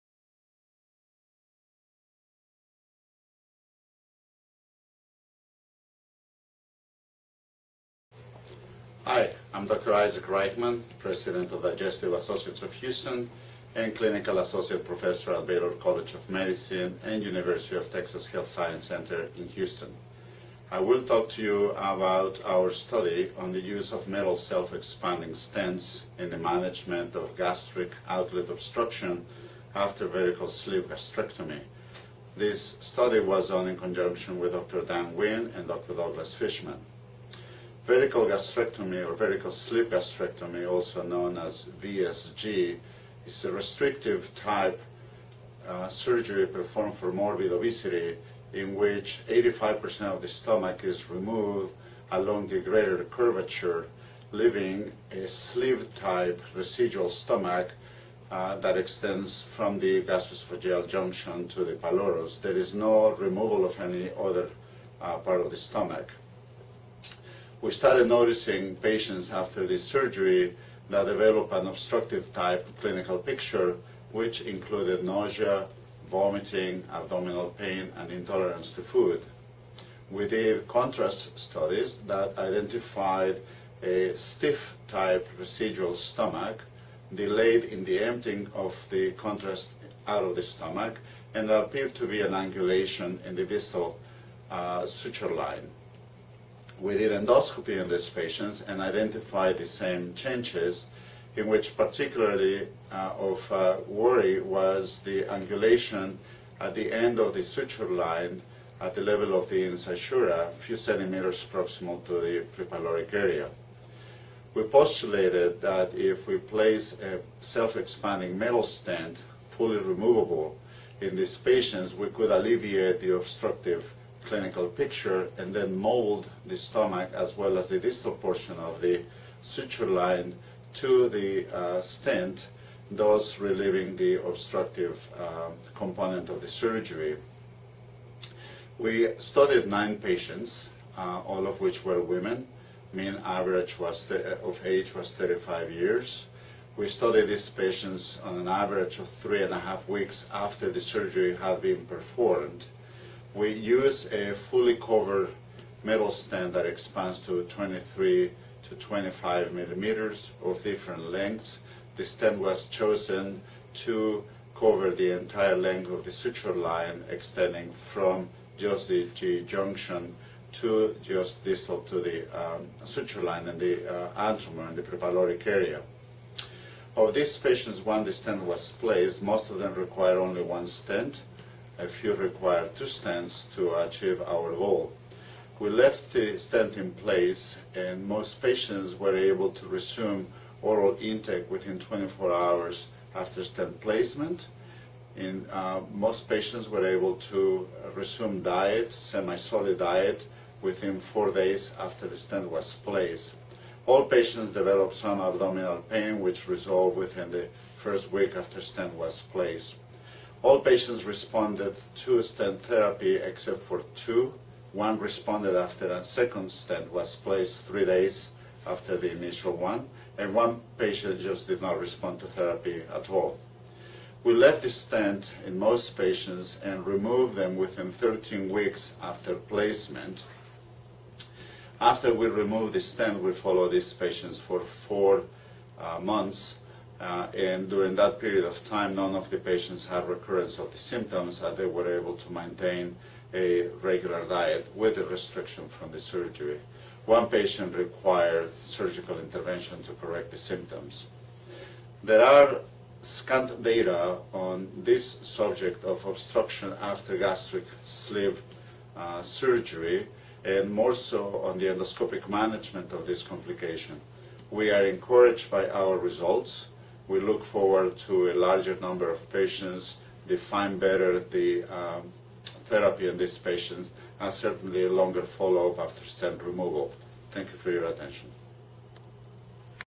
TU1560, Raijman
Use of self-expanding metal stents for gastric sleeve surgery-related gastric outlet obstruction.